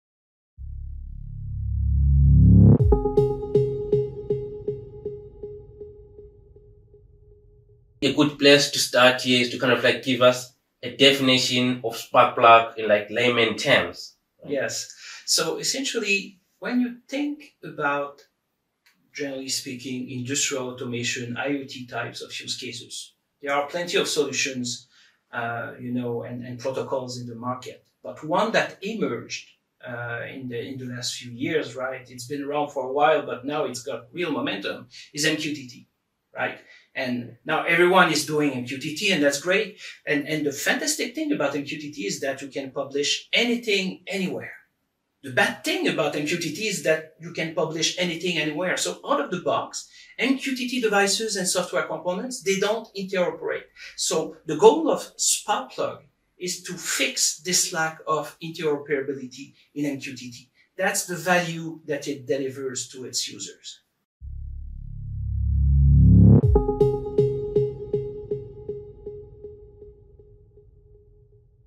What is MQTT Sparkplug?
What is MQTT Sparkplug? Get the definition of this open-source software specification for MQTT, helping the manufacturing industry to seamlessly integrate data from their applications, sensors, devices, and gateways, from the industry experts Frédéric Desbiens, Program Manager, IoT and Edge Computing at the Eclipse Foundation, and Kudzai Manditereza, Developer Advocate, at HiveMQ, in the first part of the brand new series Industry 4.0 Talks.

Resources:

- Sparkplug: The Open Specification Critical to Achieving ROI in the Industrial Internet of Things Whitepaper